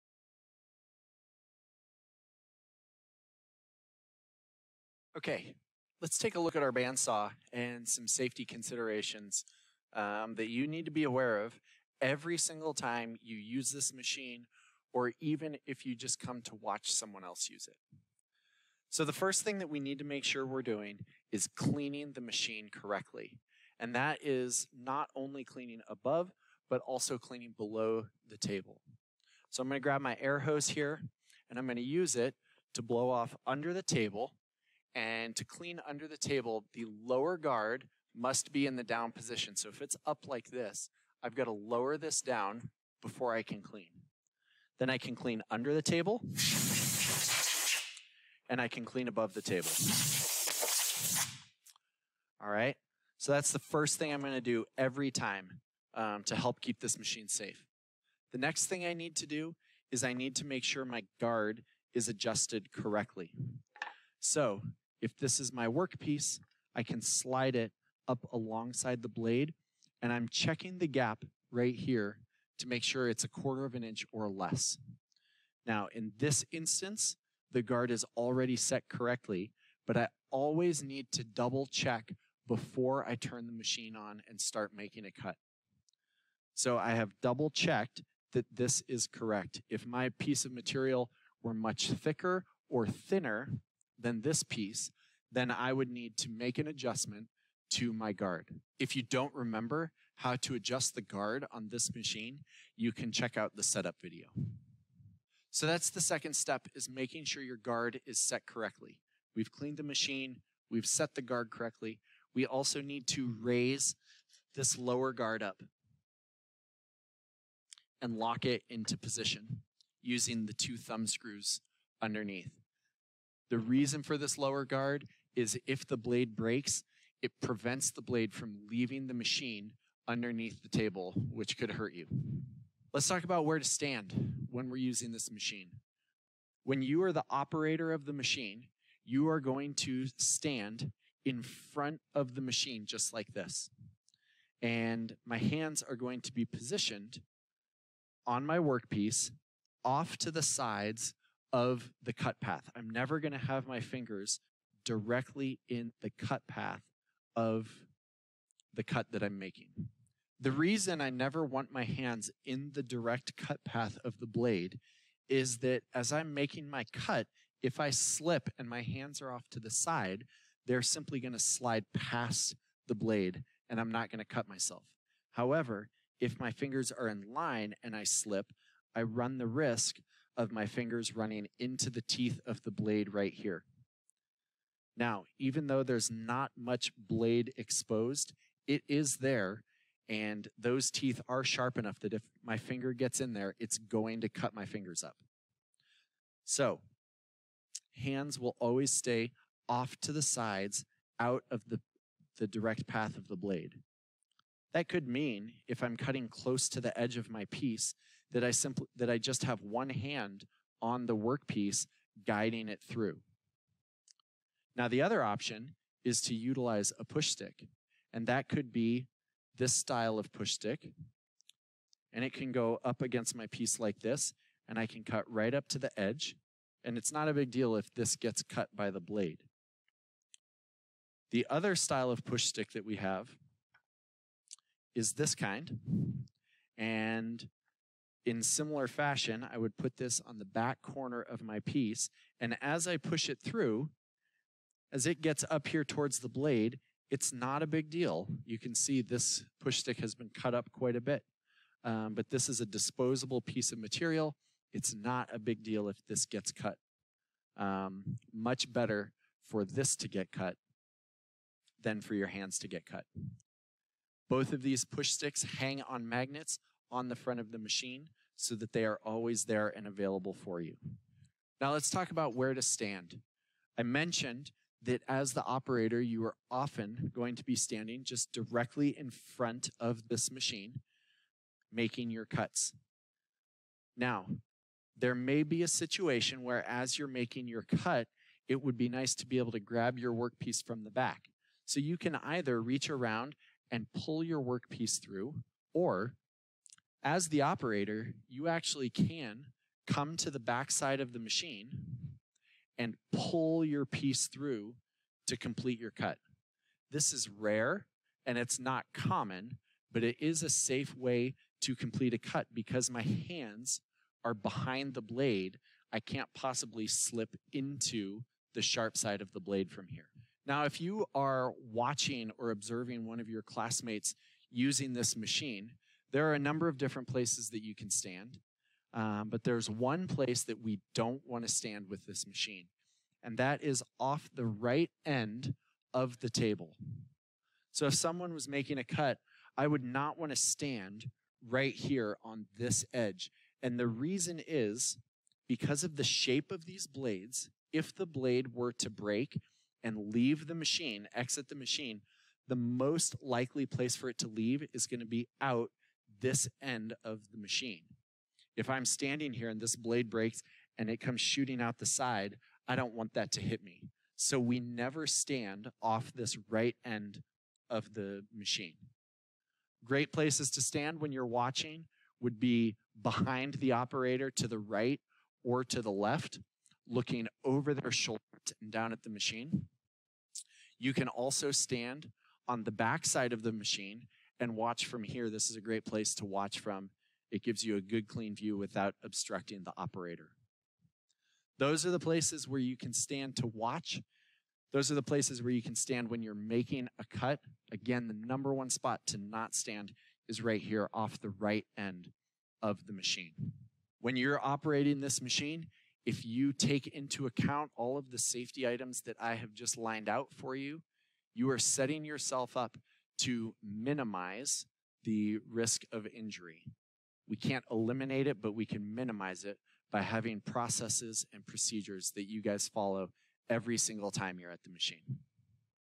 Band Saw - Safety
In this video, Taylor looks at the different safety concerns and procedures specific to the Band Saw.